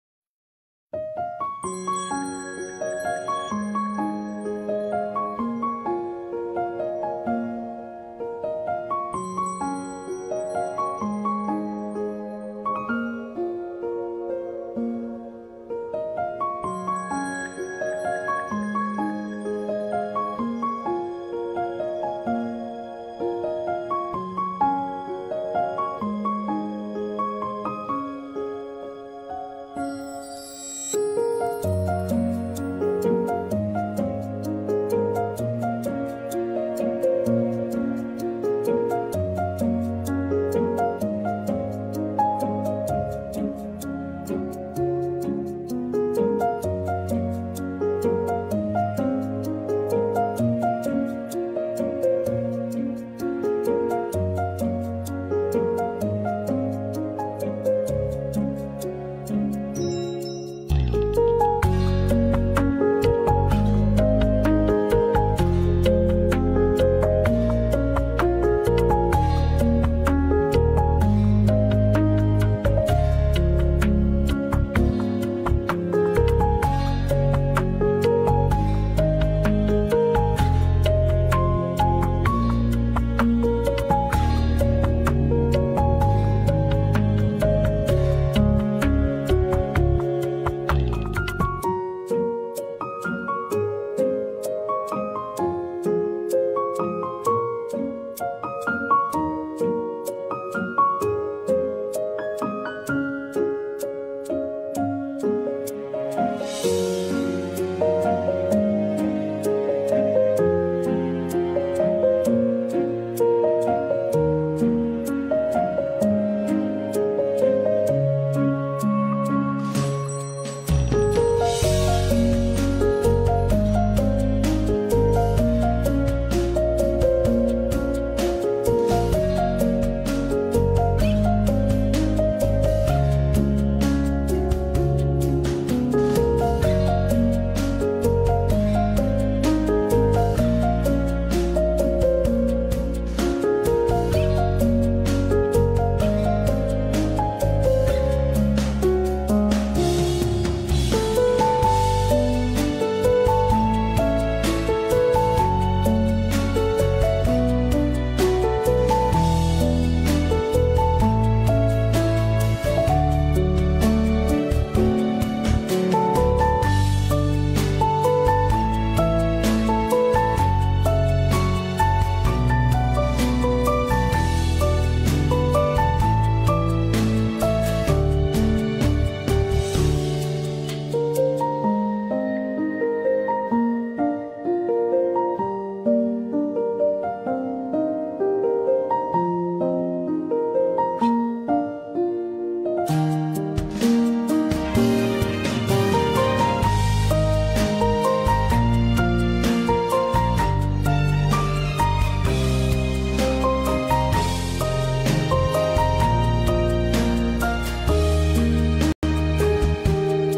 老妈做的面食怎么吃都不够？家常烙筋饼的做法 隔着屏幕看饿了！
【新红与小新】老妈做的面食怎么吃都不够？家常烙筋饼的做法 隔着屏幕看饿了！

老妈烙的饼咋吃都不够？家庭筋饼做法简单易学，真是色香味俱全！

东北大小姐：分享东北一家人的点滴生活